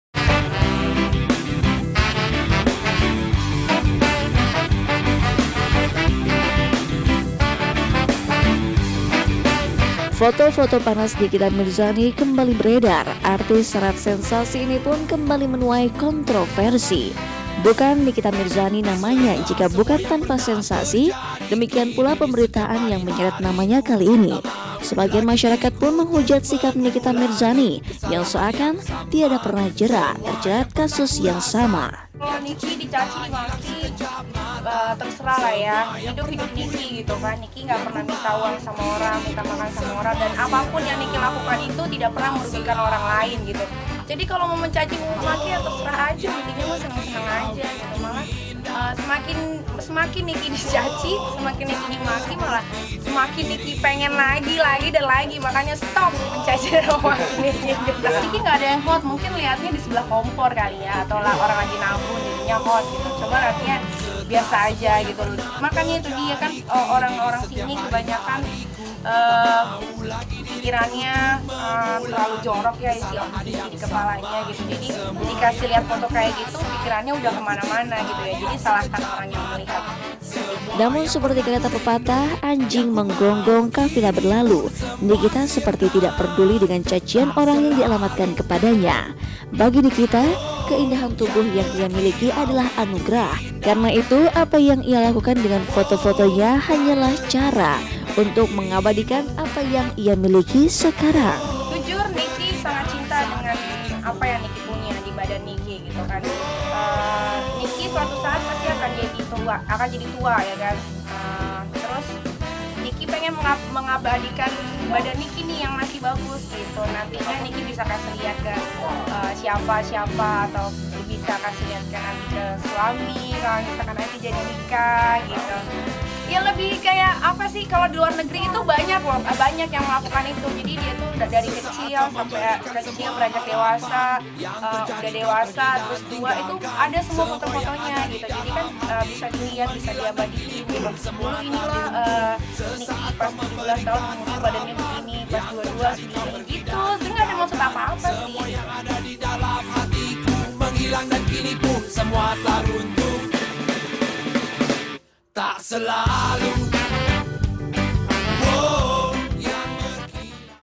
Dicaci Soal Foto Panas, Nikita Mirzani Cuek
Foto-foto panas Nikita Mirzani kembali beredar, artis sarat sensasi inipun kembali menuai kontroversi. Sosoknya kembali menjadi hujatan sebagian masyarakat, namun Nikita Mirzani tak bergeming dan terkesan cuek. Mau tahu berita selengkapnya? Simak videonya hanya di KapanLagi!&nbsp;